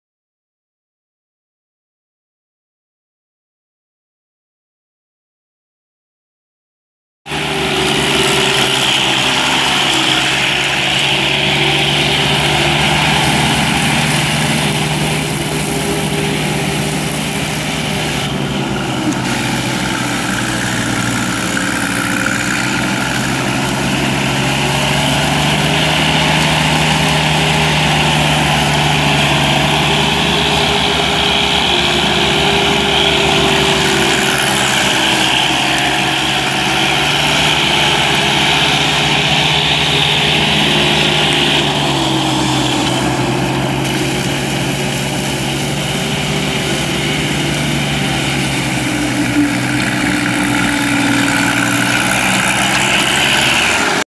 2005 Harper DewEze ATM72LC slope mower for sale | no-reserve Internet auction September 28, 2016
View a detailed listing of this item and place bids

2005 Harper DewEze ATM72LC slope mower
530 hours on meter
72" cut
Yanmar diesel engine
30 HP
Hydrostatic transmission
All wheel drive
ROPS
Power steering
Articulating center deck
Hydraulic decks
25x12.50-12 drive tires

Location
4617 South Lee's Summit Rd
Independence, MO 64055

Item location address: This item is located at 4617 South Lee's Summit Rd, Independence, MO

It's located in Independence, MO, and we're selling it to the highest bidder regardless of price.